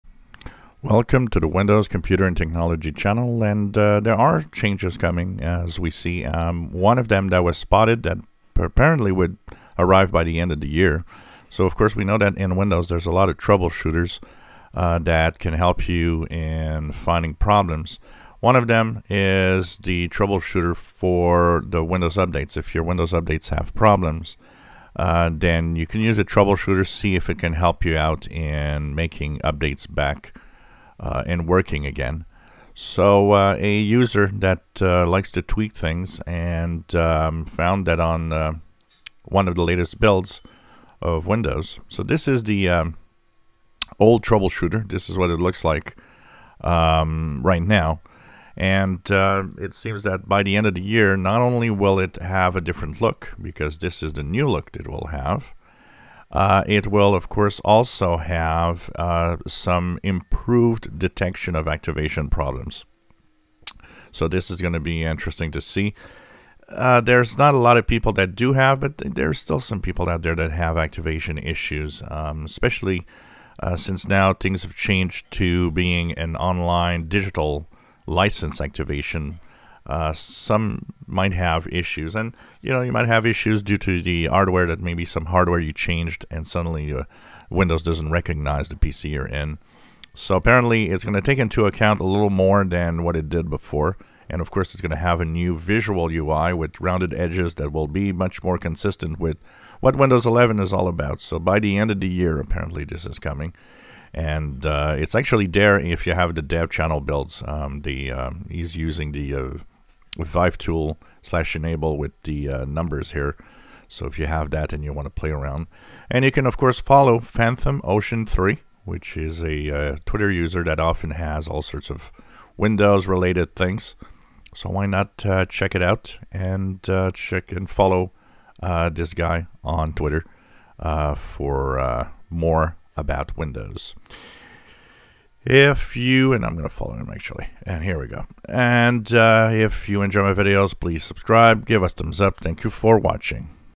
Windows 11 will get redesigned Windows Activation troubleshooter and better help options
Activation troubleshooter will have a Windows 11 feel and look coming later this year, Activation problems will also have more options to help out